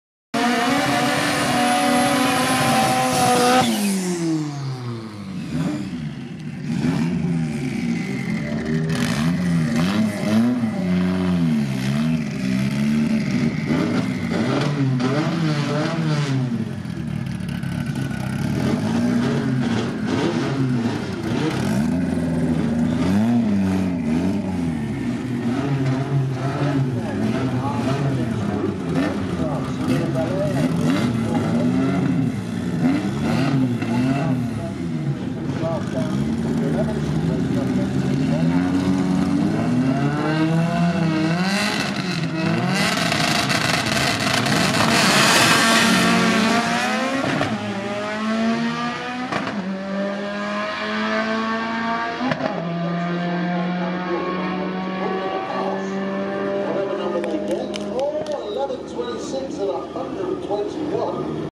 November 2021 QTM Day 2 Run 67
12th November 2021

Day 2 "Dial ins and qualifying" day of our final event for 2021.. a packed weekend with over 137 entries! lots of videos coming this weekend!